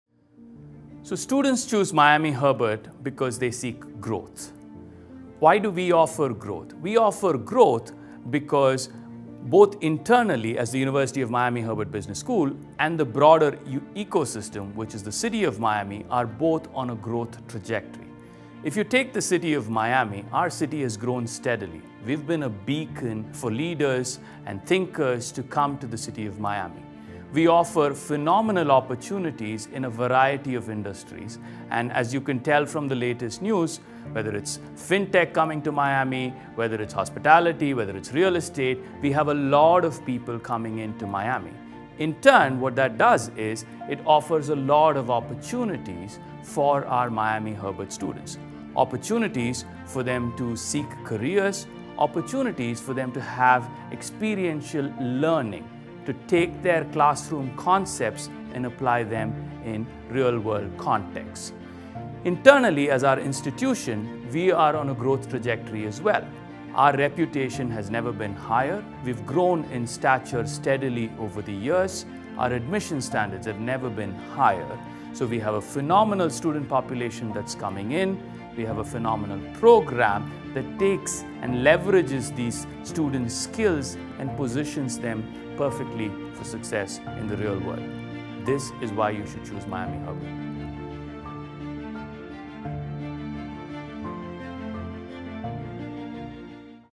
Why You Should Come To Miami Herbert Business School | University of Miami
Miami Herbert Business School ranks #1 in Florida and #36 in the nation, according to Poets and Quants for Undergrads 2021. We offer undergraduate and graduate degree programs in FinTech, entrepreneurship, accounting, marketing, and more.

Learn more about the admissions process, degree programs, campus life, students, and life in Miami